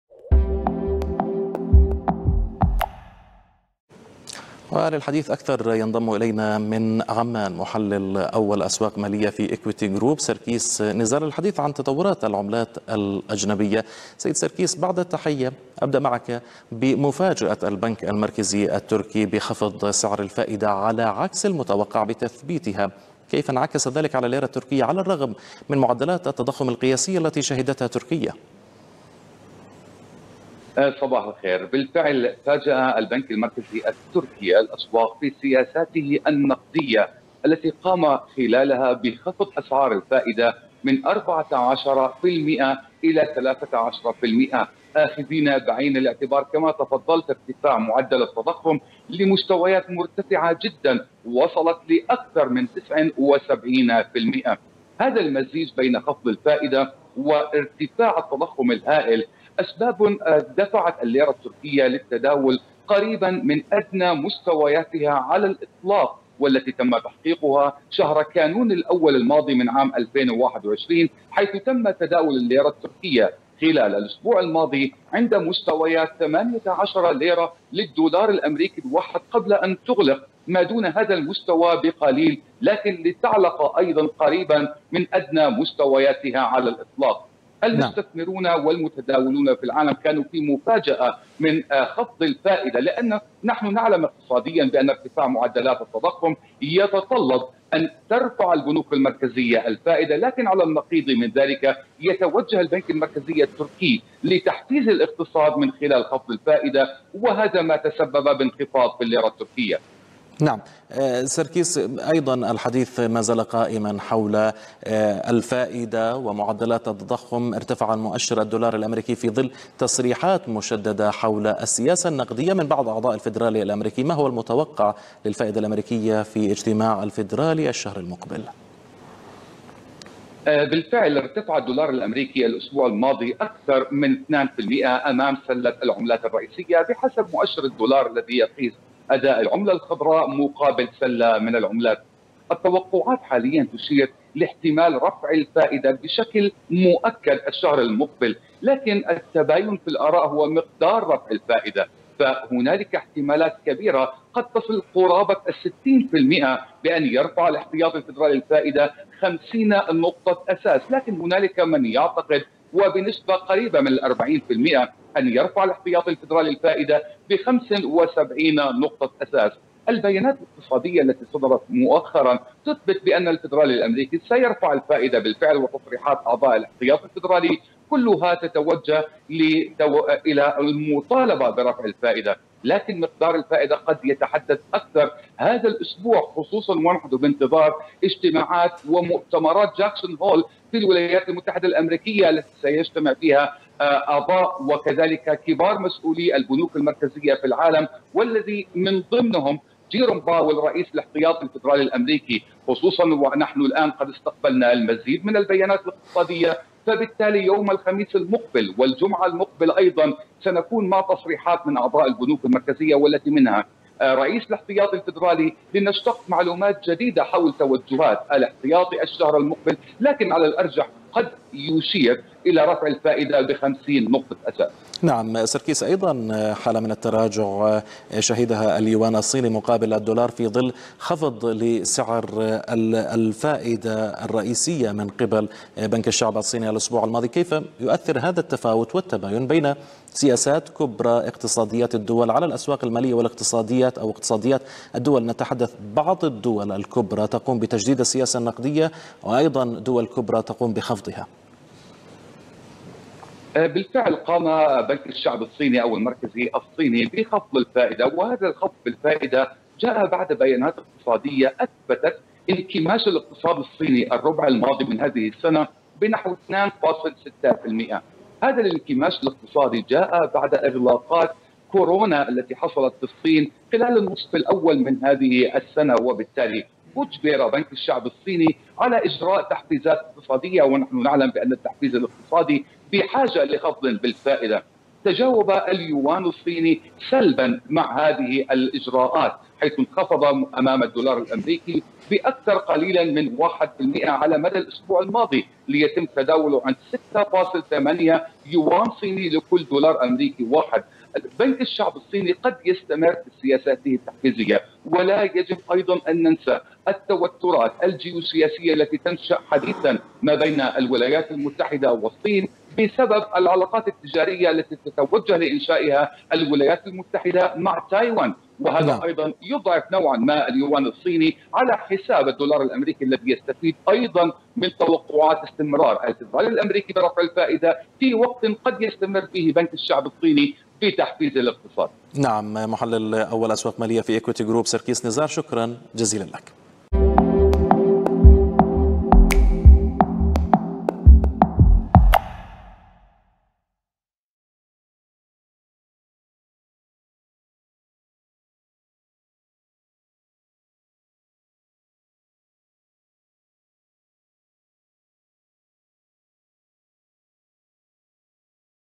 ارتفاع الدولار انتظاراً لتصريحات الفيدرالي وبيانات اقتصادية، وانهيار الليرة التركية وهبوط اليوان
التوتّرات الأمريكية الصينية تهبط باليوان فيما الليرة التركية تهبط قريباً من أدنى مستوياتها على الإطلاق وسط استغراب الأسواق من خفض الفائدة في البنك المركزي التركي.
وهذا الأسبوع، تصريحات الفيدرالي والبيانات الاقتصادية المنتظرة قد تكون سبباً لتحرّك ملموس بالدولار الأمريكي.